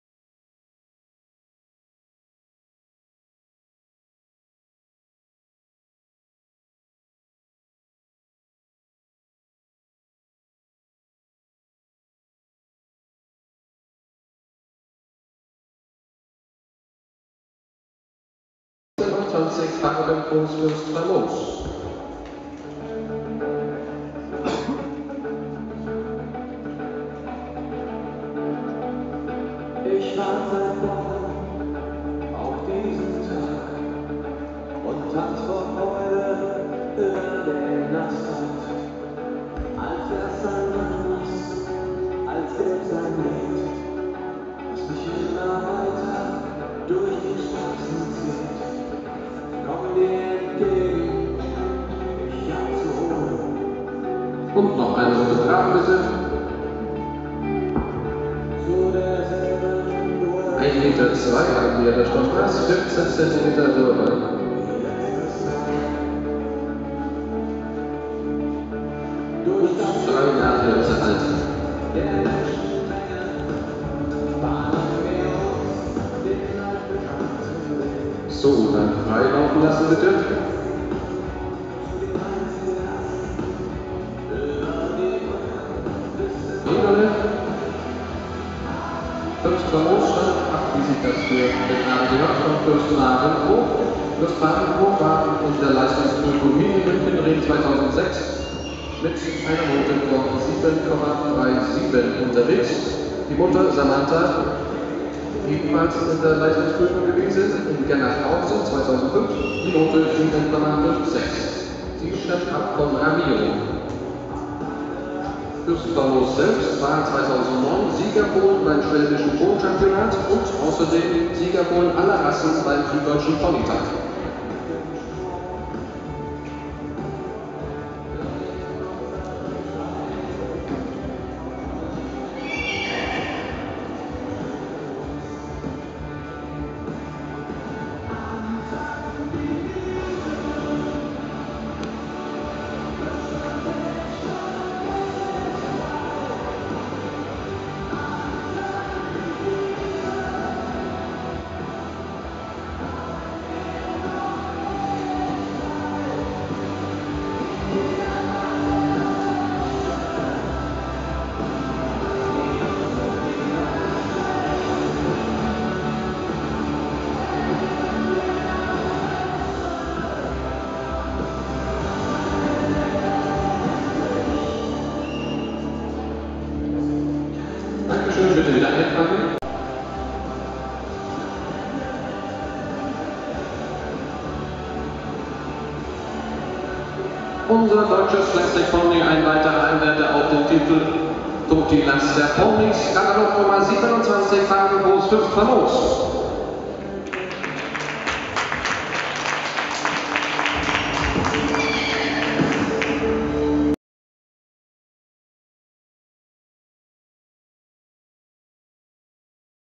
AVC.Dt Classic Pony-Hengst „Farbenfrohs Fuerst Famos".2012
Bayerische Ponyhengstnachkörung in München im Oktober 2012: Bewegungsstudien und Körurteil von Dt. Classic Pony-Hengst „Farbenfrohs Fuerst Famos" aus dem Ponygestüt „Farbenfroh" von Artur Nieberle,